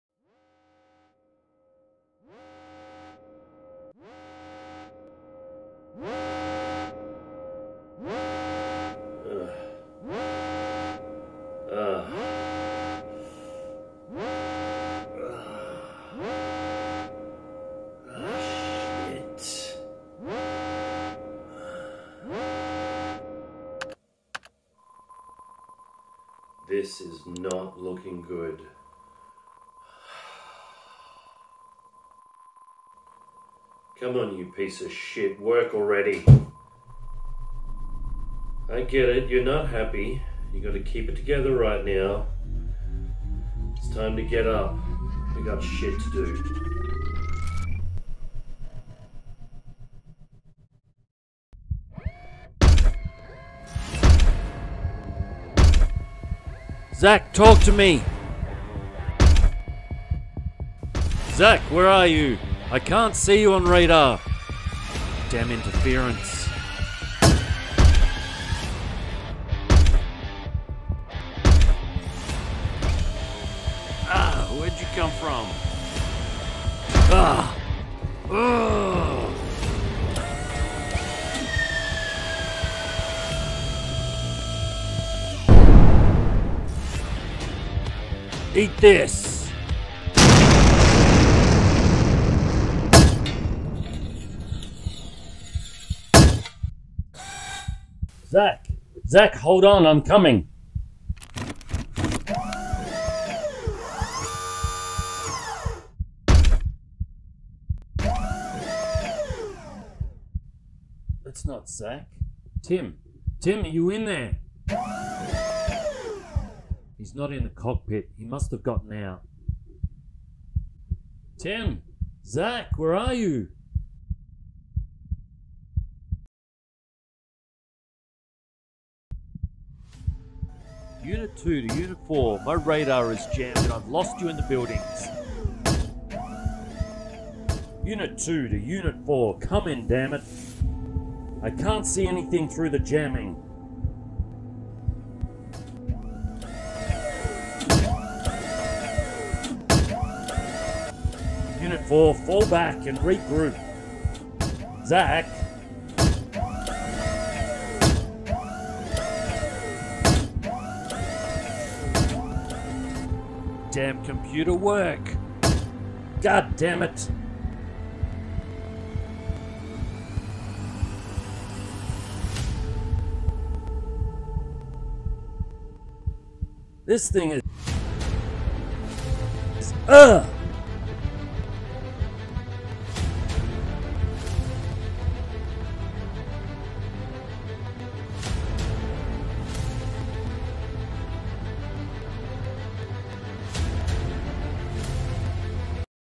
Brother's Keeper - Made in Blender
So there comes a time when you can either keep making stuff because you think people will like it or make something for yourself.
So, I made a start on 'Brother's Keeper'
I made a few starts in other programs but wasn't getting a polished look, so I made myself learn Blender, and oh my god could you have made this program any more complicated. I'm so luck there are so many helpful videos on YouTube otherwise I could have never stuck at it.
I have now finished the first intro piece that truly shows what I can do, and I think it's great!
for some comparisons I have included so of the early drafts from Adobe character animator and a blender trial vid at the end of this video.